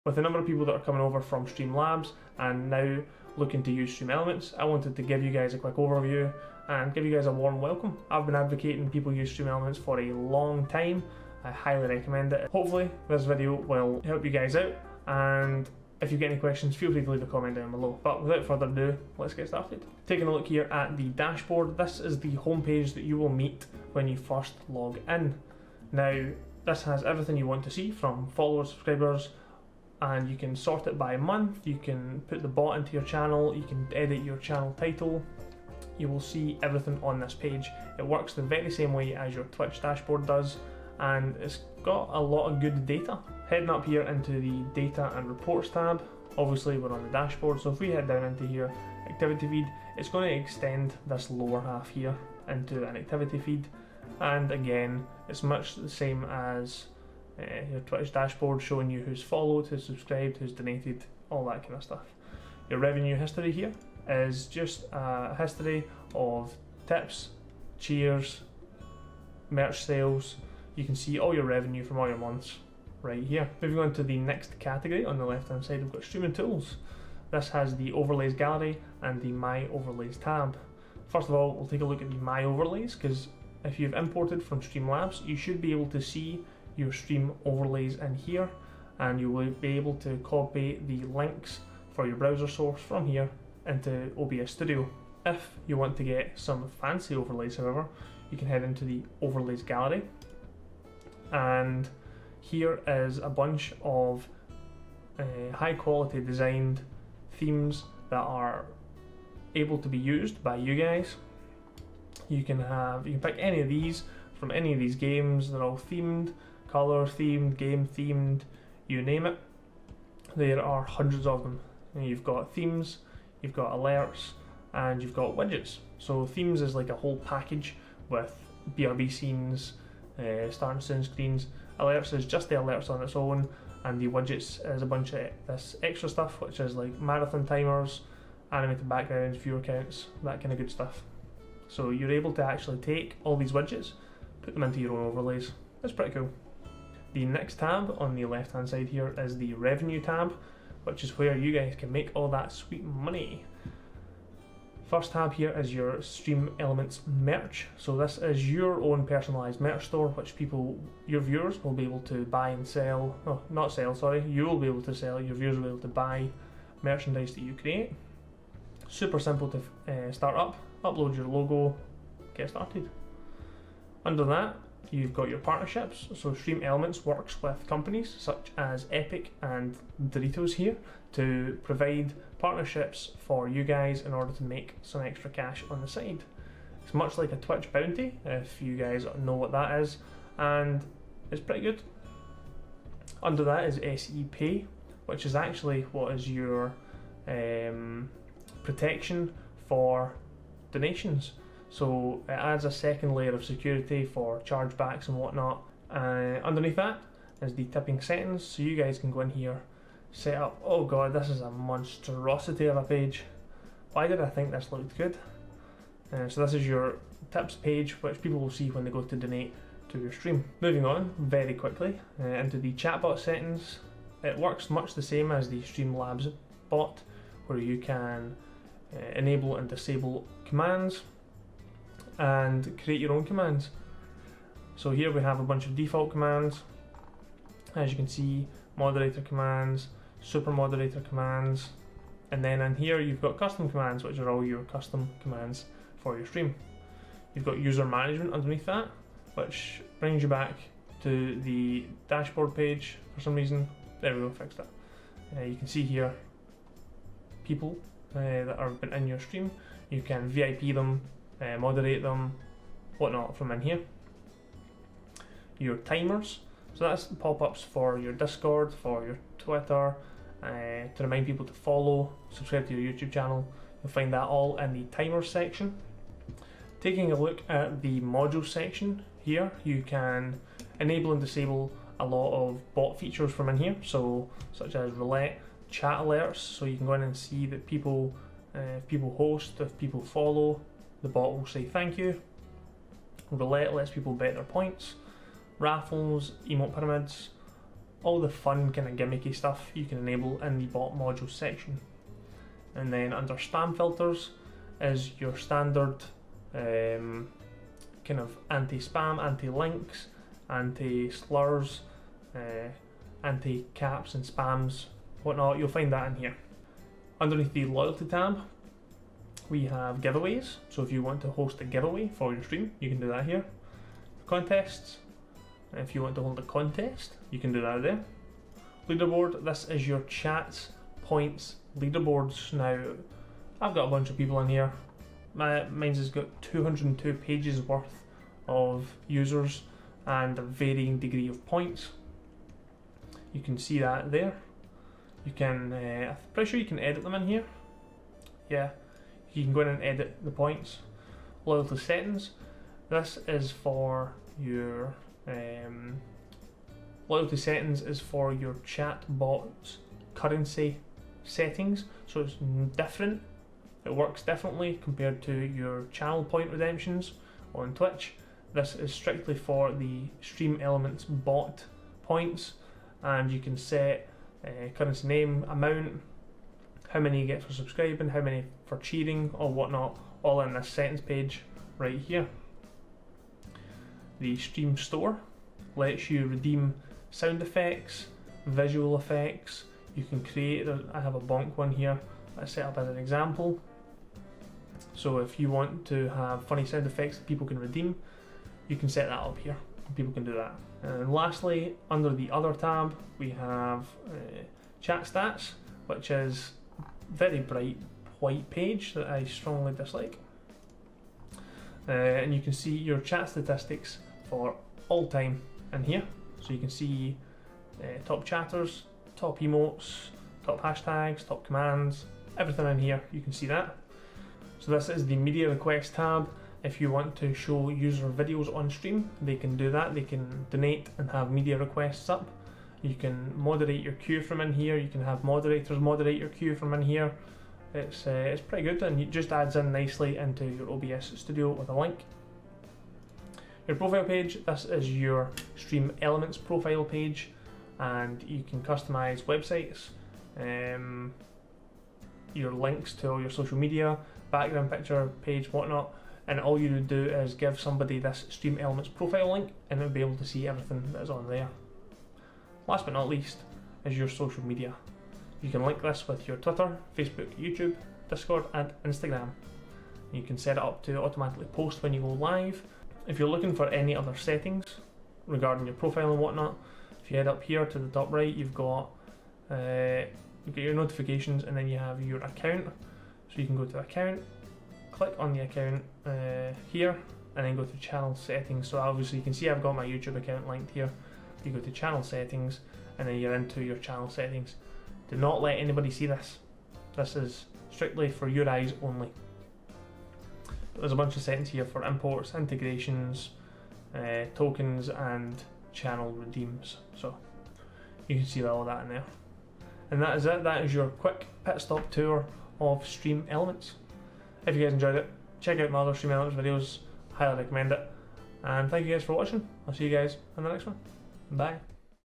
StreamElements 2021 Quick Guide!
LINKS AND TIMESTAMPS BELOW!

Quick pitstop tour of StreamElements and where to find all the important information for your stream! Covering all sections of the StreamElements main page.

~TimeStamps~
-00:00 Data & Reports
-01:10 Streaming Tools
-02:25 Revenue
-03:40 Chat Bot
-05:12 Loyalty
-06:35 Other